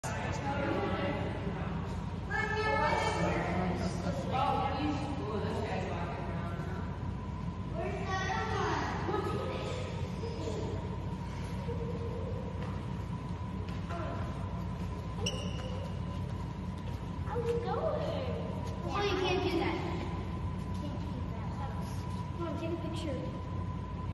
Simba is Awake
After a well-deserved nap, Simba decided it was time to explore and say hello to his visitors.